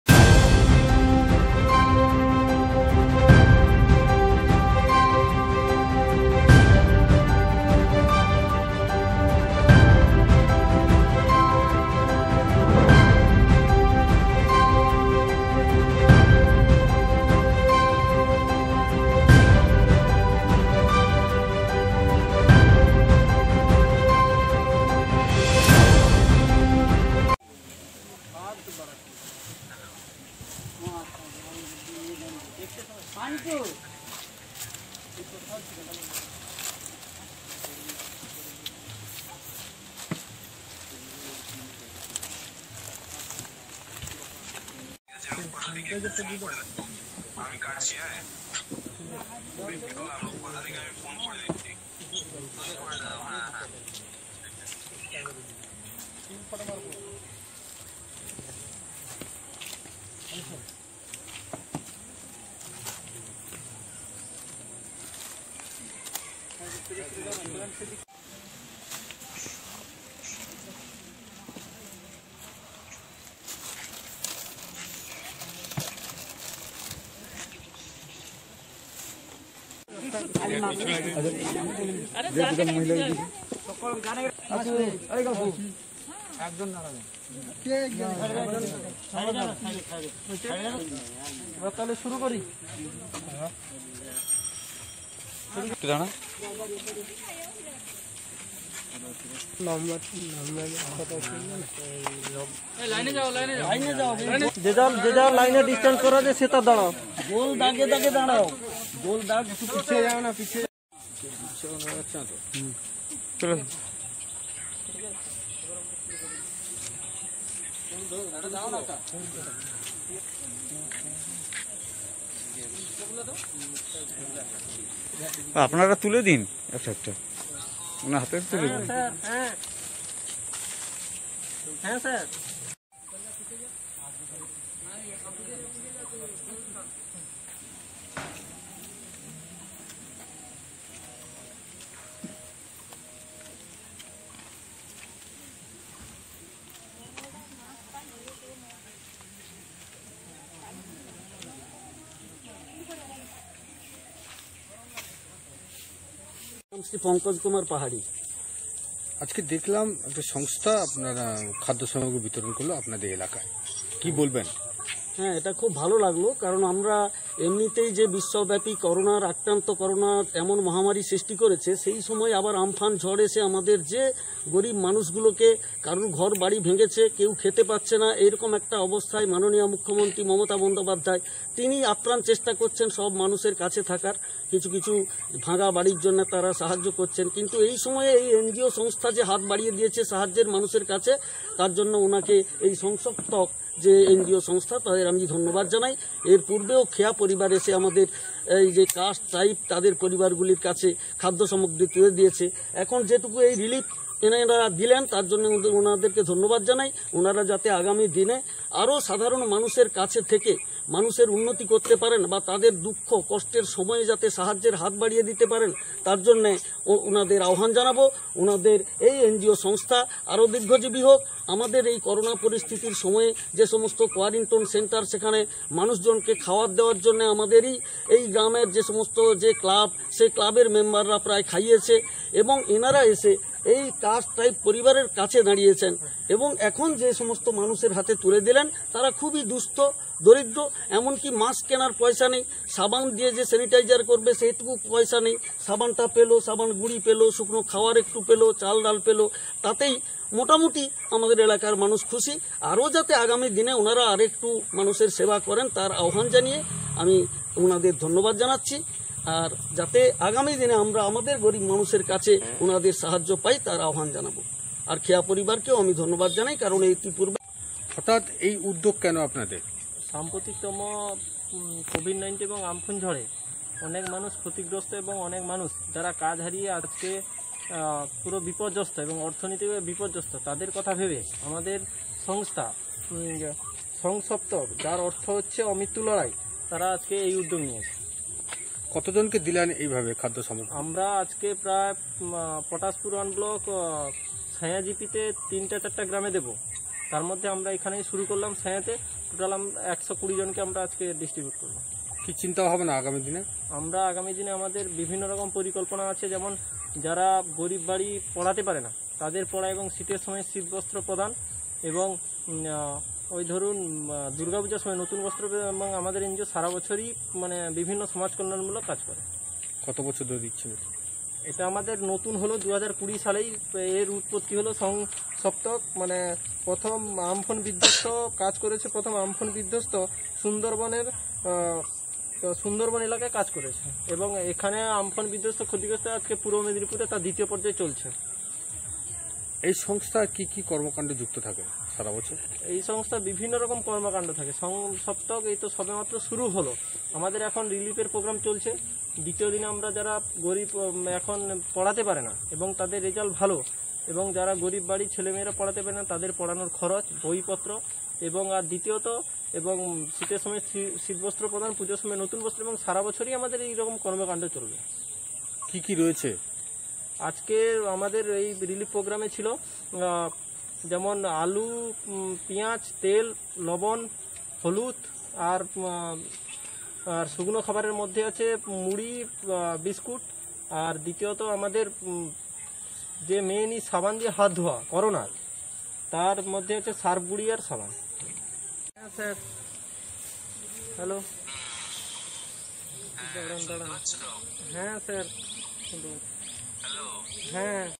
PATASHPUR 2 SONGSPTOK UDDOGE KHADDO SAMOGREE BITORON#19 07 2020 P3 NEWS BANGLA#
পটাশাপুর ১ ব্লকের ।২ নং নৈপুর গ্ৰাম পঞ্চায়েত এলাকায় সঁঞ‍্যা গ্ৰামে সংশপ্তক সংস্থার
পক্ষ থেকে অসহায় দরিদ্র মানুষের হাতে খাদ্য সামগ্রী তুলে দিলেন।এই অঞ্চলের পঞ্চায়েত সদস‍্যা ভারতী পাহাড়ি ও সংশপ্তক সংস্থার সদস্য বিন্দরা।৫০ জন মানুষের হাতে।